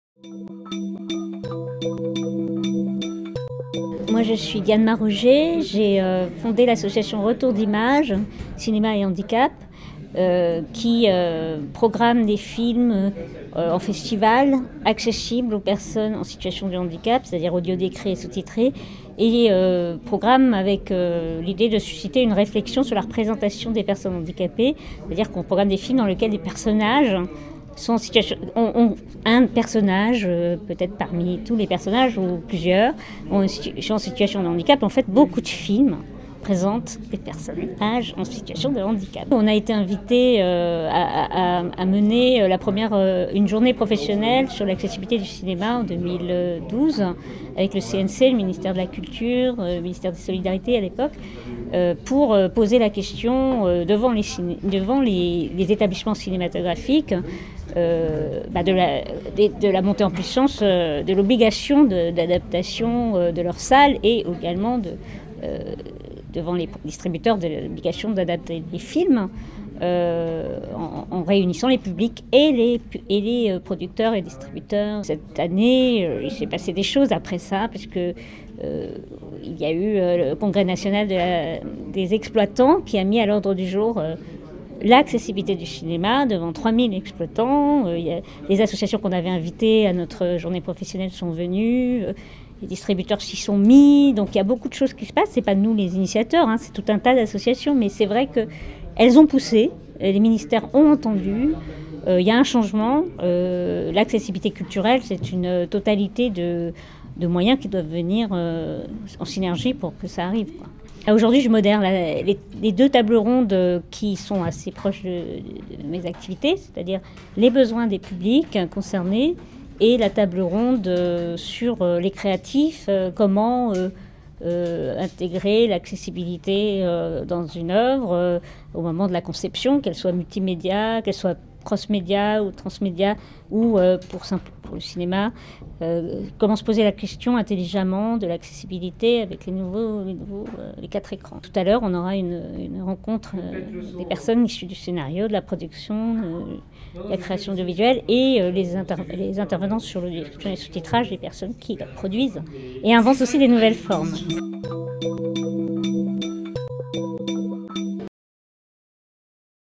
Conference2013 media4D: Diane Maroger
Conference2013 media4D: Le contenu visuel face à l'innovation technologique. Interview de Diane Maroger - Fondatrice Retour d'Image .
Conference du 29/03/2013 à Plaine Commune organisé par Holken Consultants & Partners

Media4D Conference 2013: Visual Content meets Technological Innovation. Interview with Diane Maroger - Founder Retour d'Image .
Conference :  03/29/2013 in Plaine Commune organized by Holken Consultant & Partners